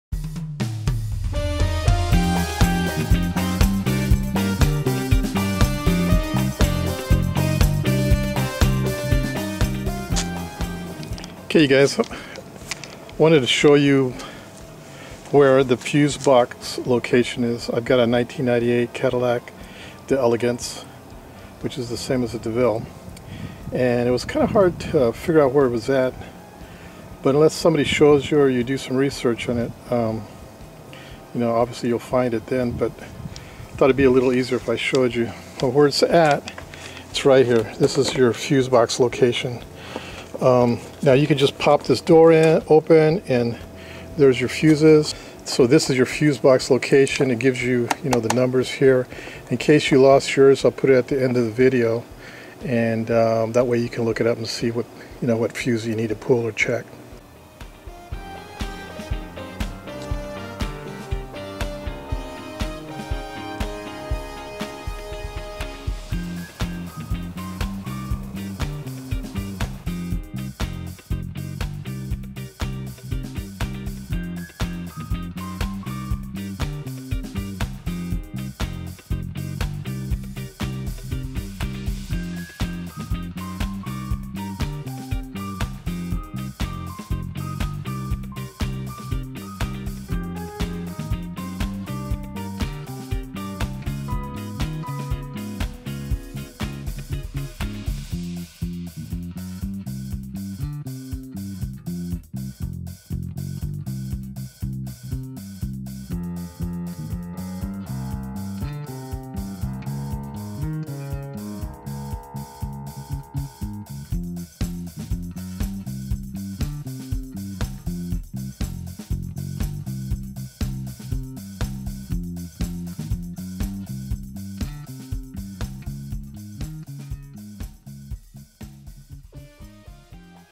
1998 Cadillac Deville - 1998 Cadillac Deville problems - wheres my fuse box
Where is my fuse box 1998 Cadillac Deville. After doing some research, about my 1998 cadillac problems with my fuse box, I finally figured out where my fuse box is so I thought I'd save you some time and make a video and show you exactly where it's at.  this is on my 1998 Cadillac d'Elegance, DeVille. I hope this video helps you.

PayPal ( Support/Donations, Pay it forward to continue producing how-to videos to benefit others )